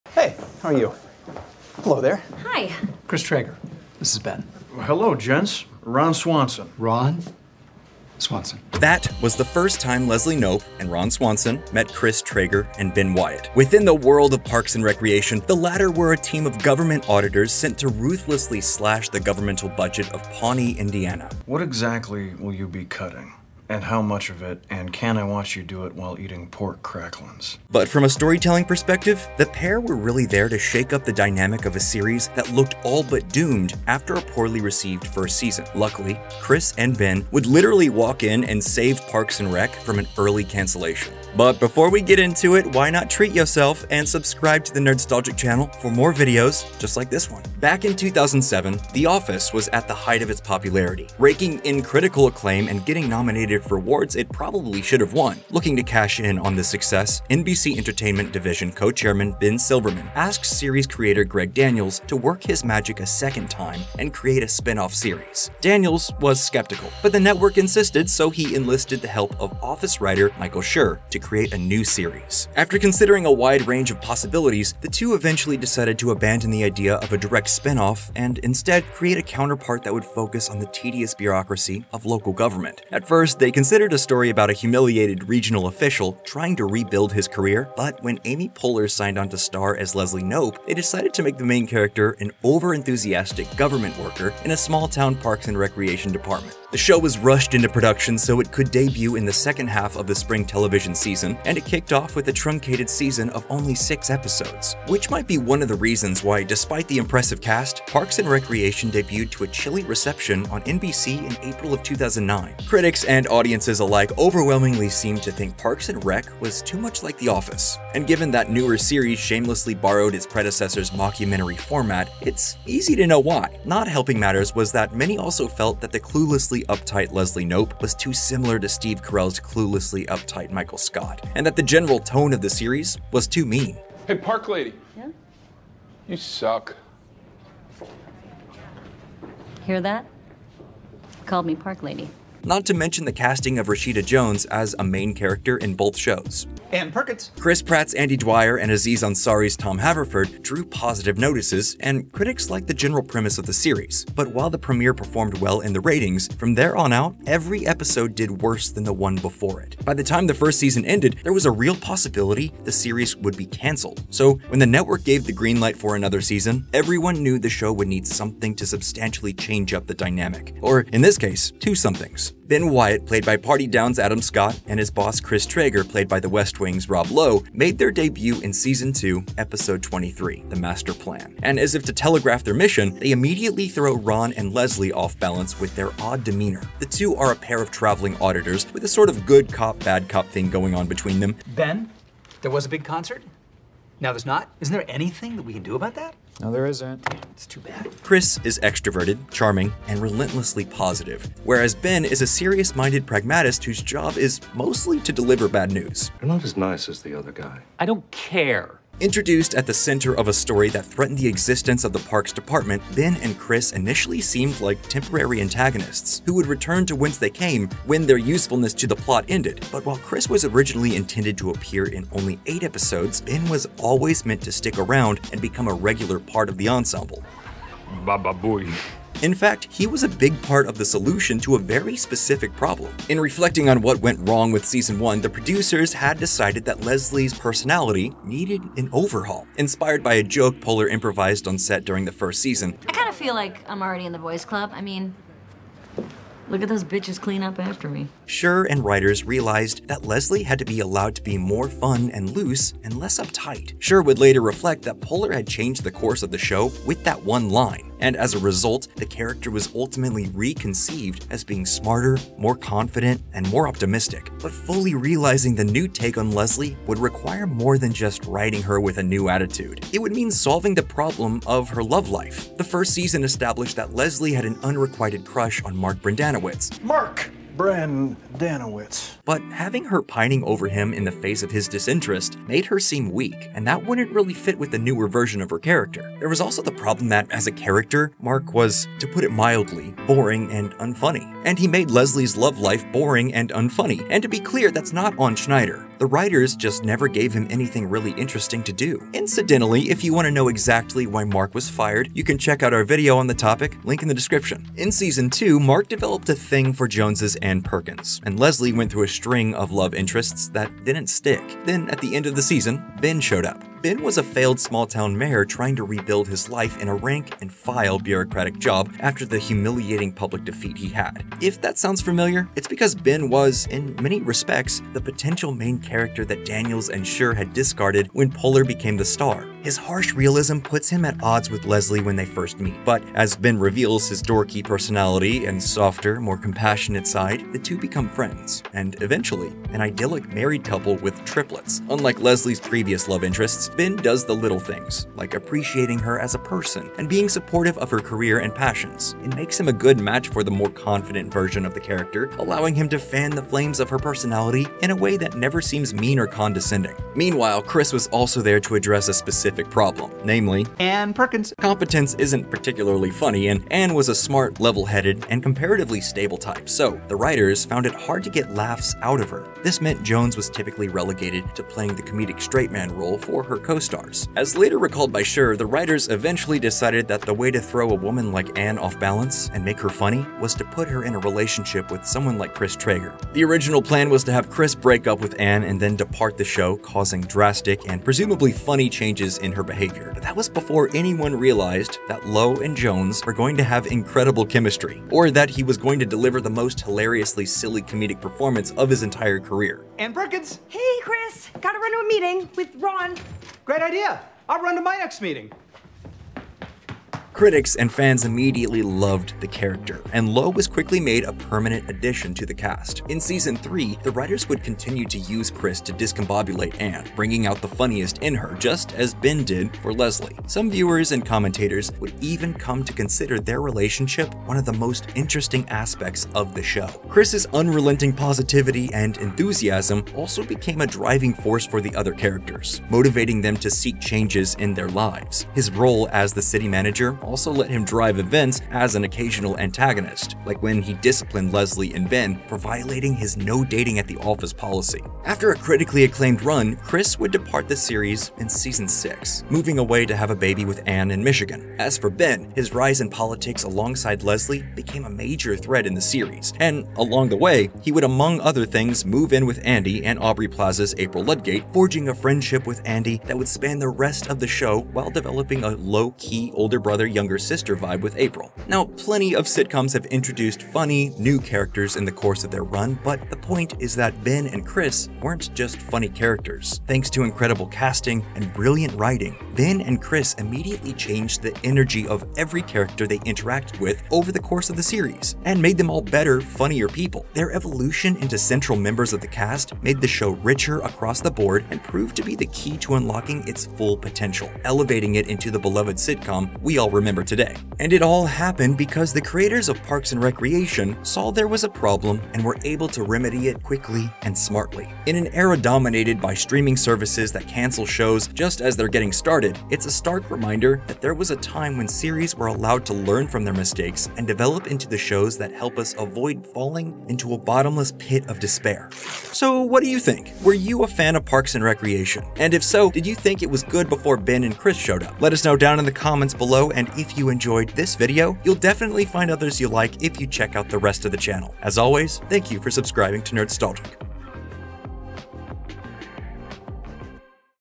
How Two Characters Saved Parks And Recreation From Cancelation
After it's first season, Parks and Recreation wasn't off to the best start.  It was panned for it's similarities to The Office, and overall didn't live up to the hype of it's predecessor.  But at the end of season two, Ben Wyatt and Chris Traeger were introduced and Parks and Rec would never be the same.  It's rare that a sitcom can completely save it's self by adding to the central cast, but shows like The Office and Parks and Recreation took the gamble and it paid off.

The Greatest TV Characters Who Weren't Even In The First Season